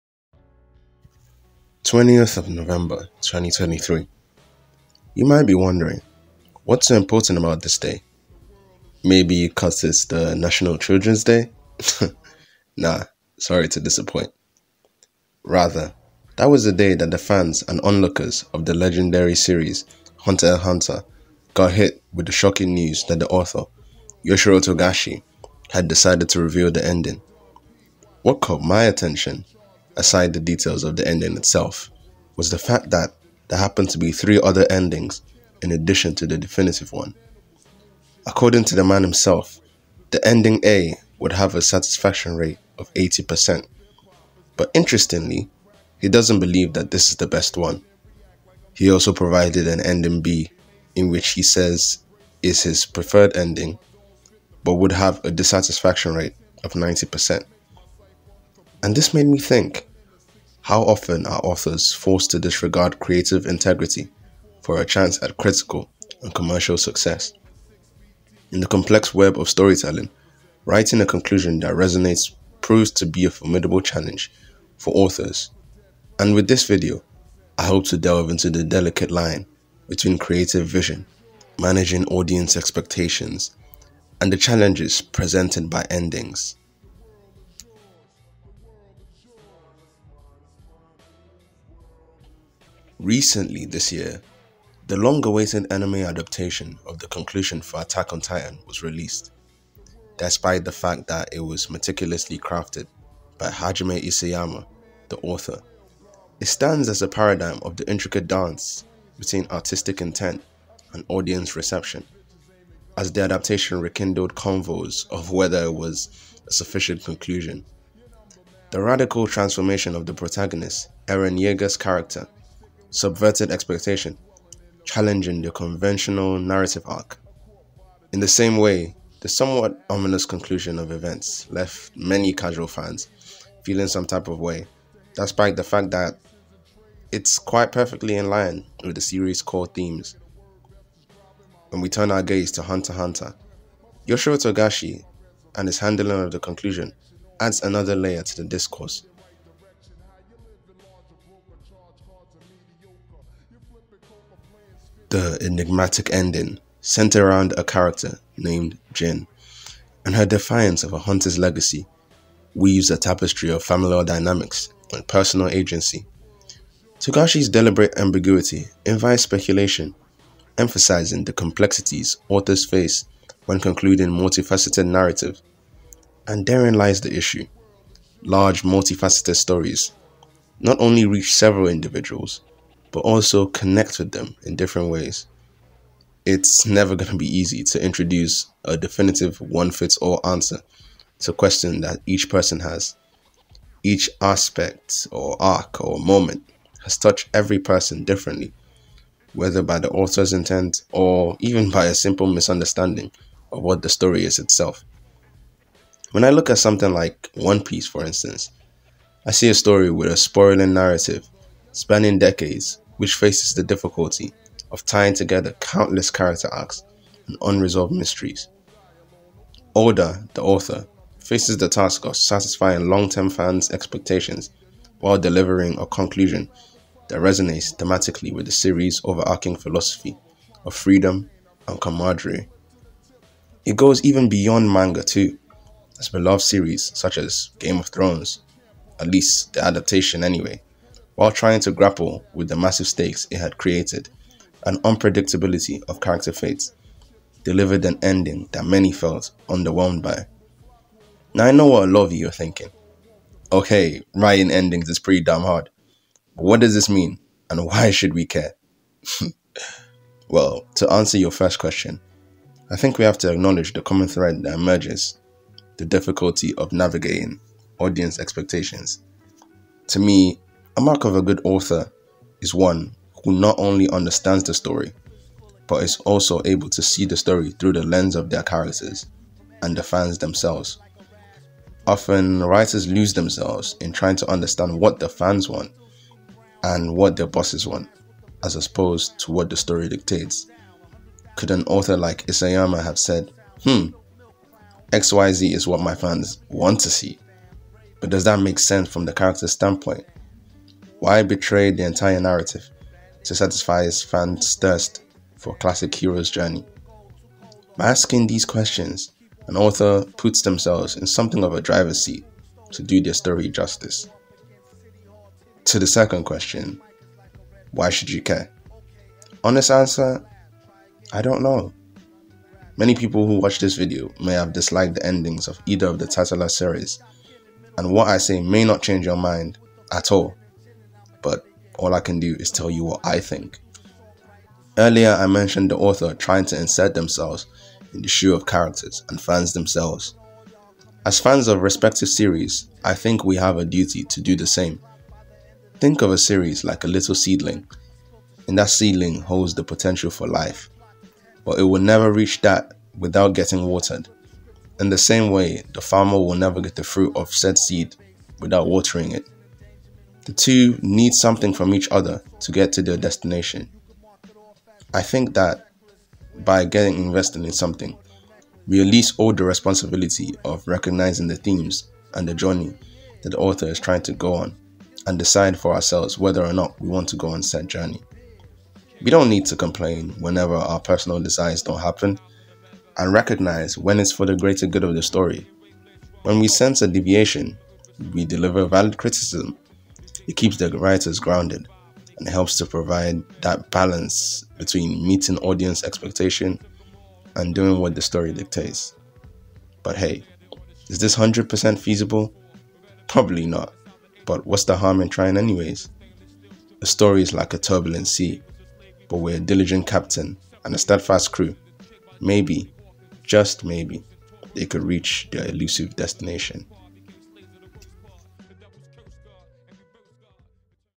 How Attack on titan and HxH show the difficulty of writing endings
Video got blocked in Russia of all places lol, but we move.

Making this video would be impossible without them